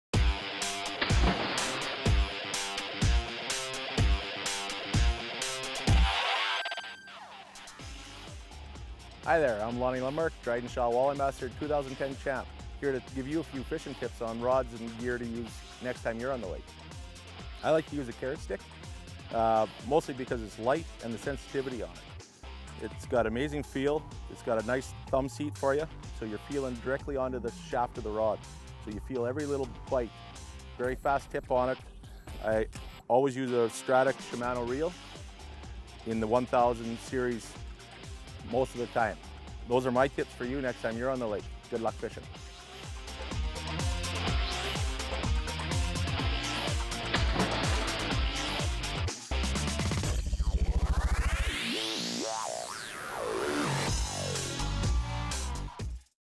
Walleye Bites 2011 episode 6
In the sixth episode, we discuss the fishing rod and reel.